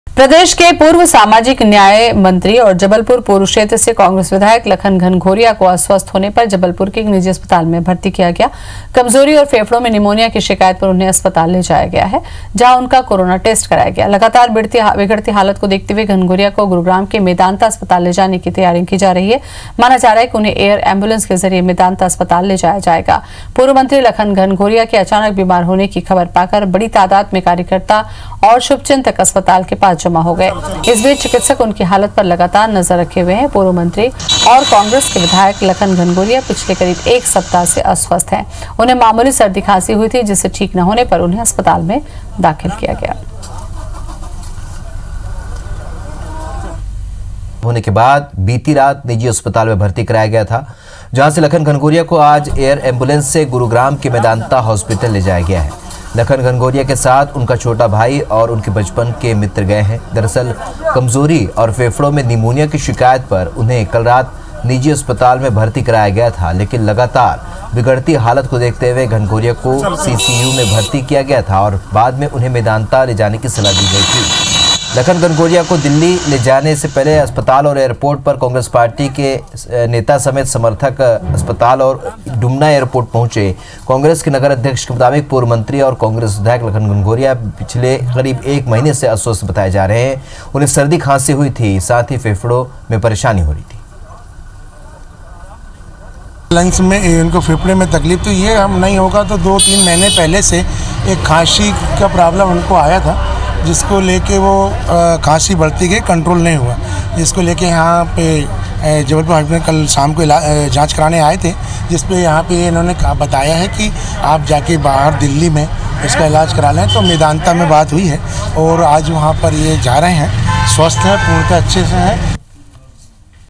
Lakhan Ghanghoria Private Hospital में भर्ती | Corona Test कराया गया
IBC24, Lakhan Ghanghoria Private Hospital में भर्ती, Corona Test कराया गया